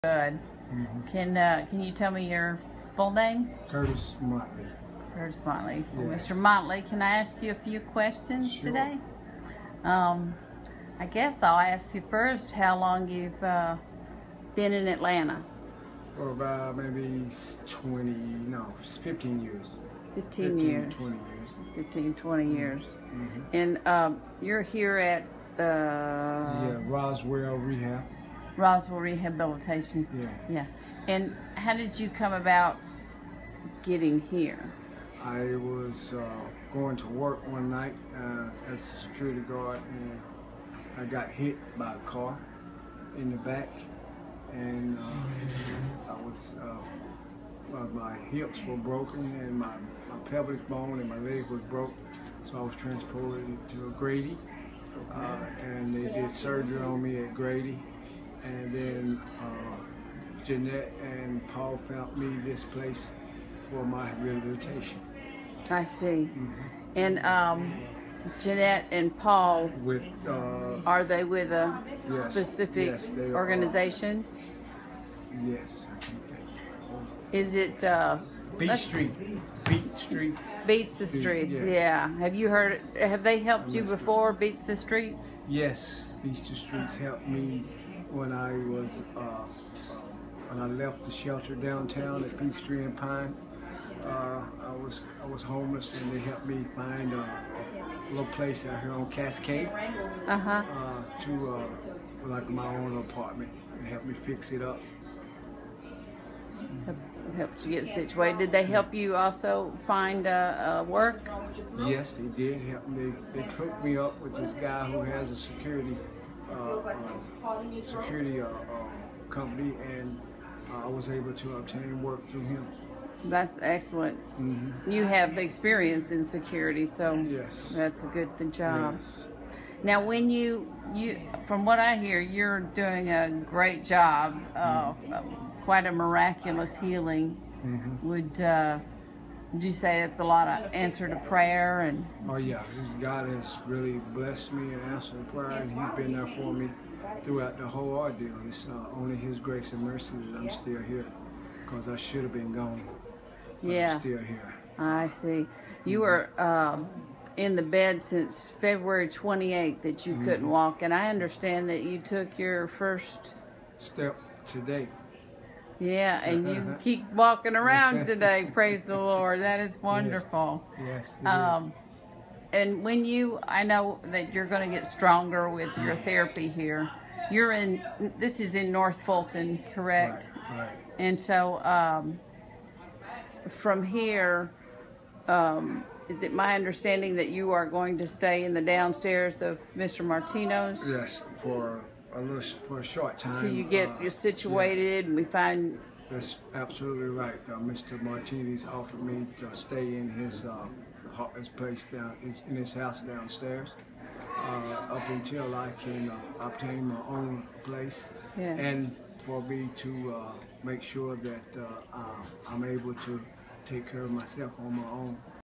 Curtis, homeless in North Fulton
Curtis was homeless for 7 years. He stayed at a shelter in the Atlanta area for much of that time. BeatsTheStreets helped Curtis transition into an apartment and get a job as a Security Officer. Six months later on February 28th, Curtis was struck by a car on his way to work. The car didn't stop and Curtis was left for dead. However, God sees all and a nearby neighbor saw the accident and called 911.
Curtis is in a North Fulton Rehabilitation Center where he is working hard to regain his ability to walk without assistance. Curtis will once again be homeless when he leaves the North Fulton Rehab Center. @PaulMartino, the CEO of BeatstheStreetsplans to have Curtis stay in the downstairs of his home until Curtis is able to find work and affordable housing in North Fulton.